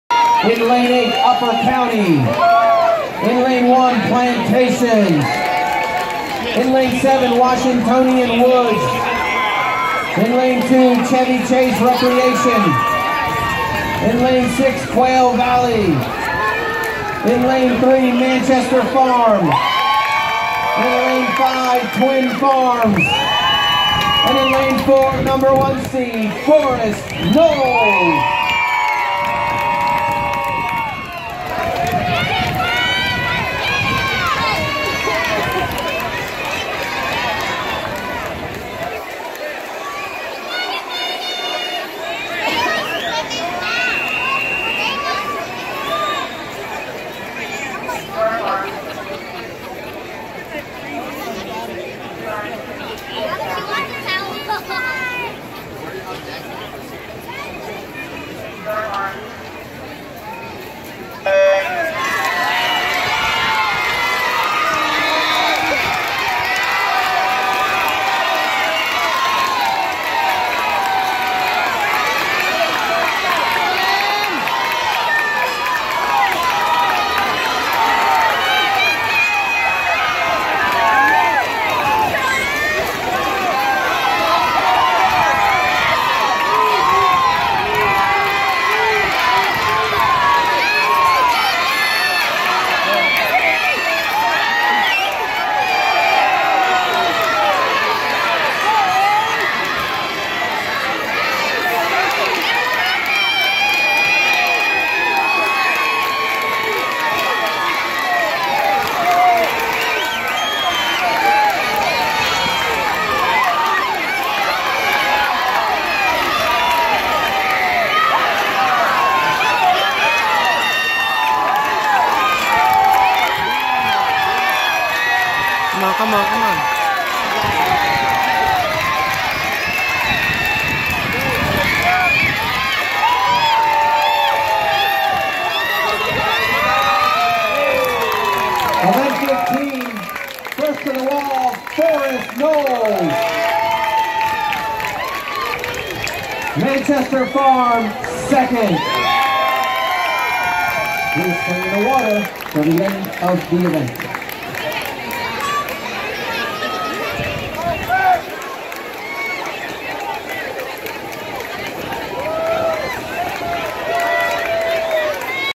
Girls 9-10 100m Medley Relay
2017 MCSL All-Star Relay
Quail Valley in Lane 6